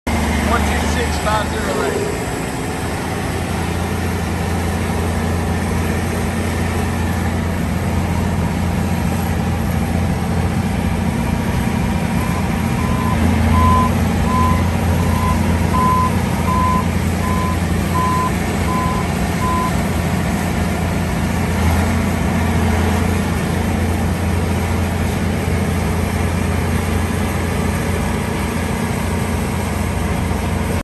JOHN DEERE 9630 For Sale
CAB, MFWD, FRONT & REAR BOLT ON DUALS, FRONT WHEEL WEIGHTS, 6 SETS HYD REMOTES, SN- 1RW9630EPBE024571 **SCRAPER SPECIAL**